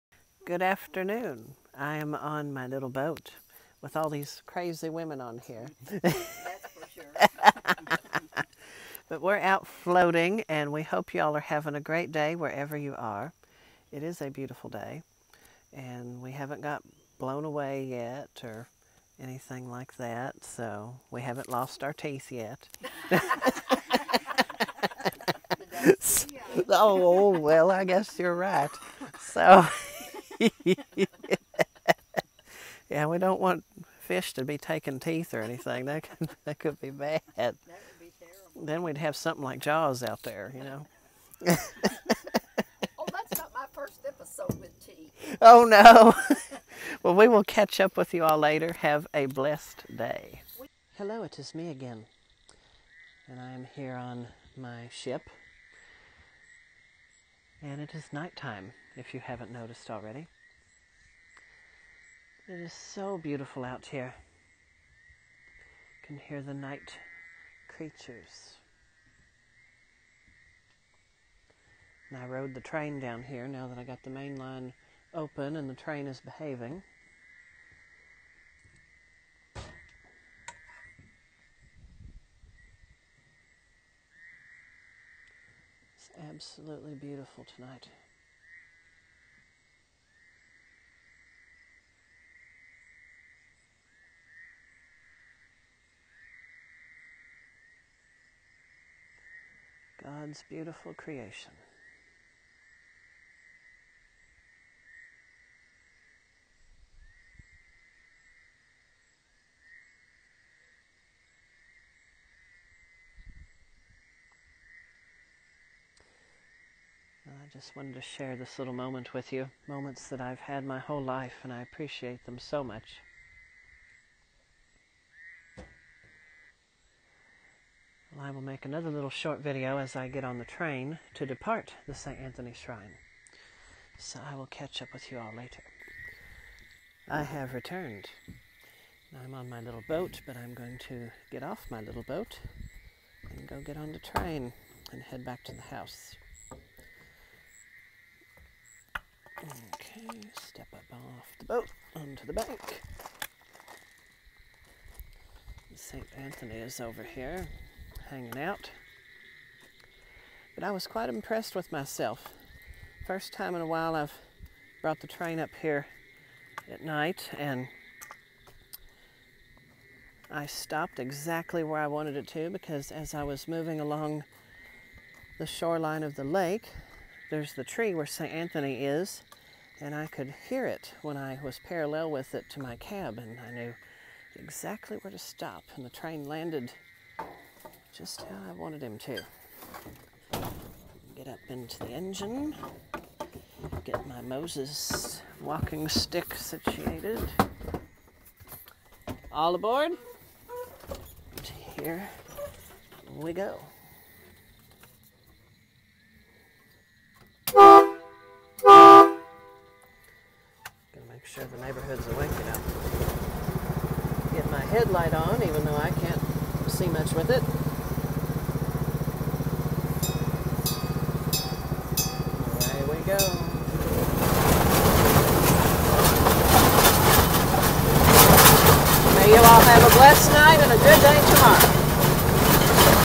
On the waves and rails
The video begins with my outing with parishioners on my boat, and then the video ends as I make the last train trip of the day at night.